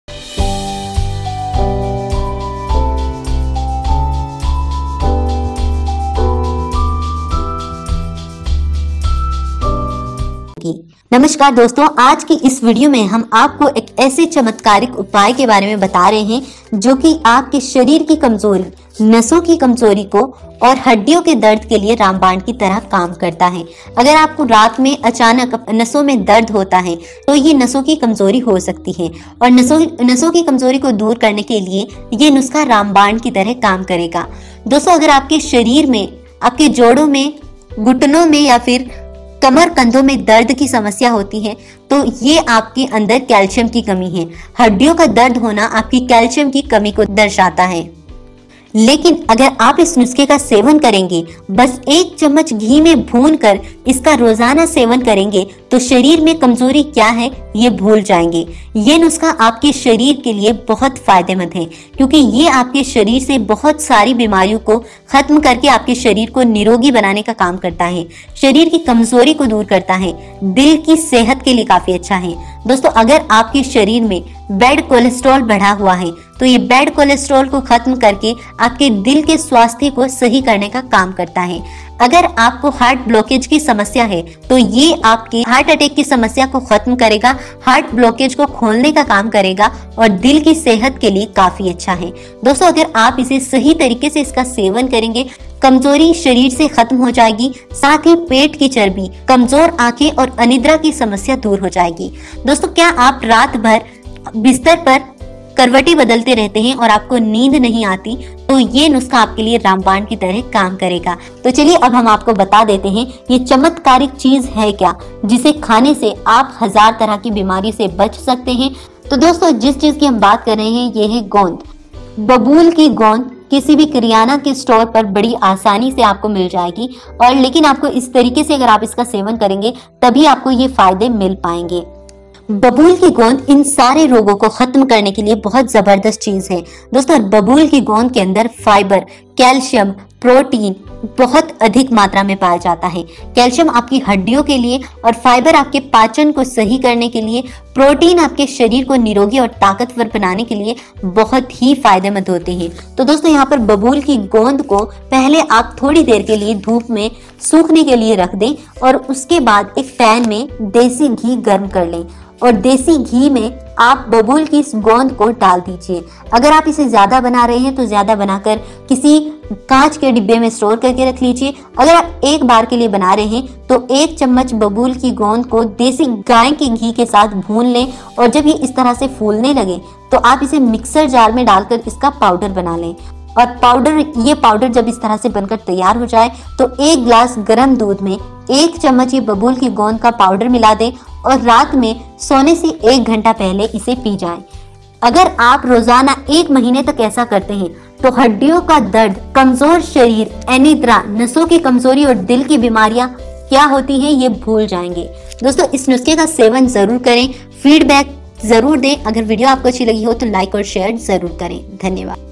शरीर की नसों में दर्द, कमजोरी Joint Pain | हड्डियों में कमजोरी |Calcium कभी नहीं होगी Vitamin d3 b12
shareer se naso ki kamjori haddiyo ka dard or calcium ki kami ko poora krti hai - Pain in the nerves of the body, weakness Joint Pain | Due to the removal of osteoporosis and the home remedy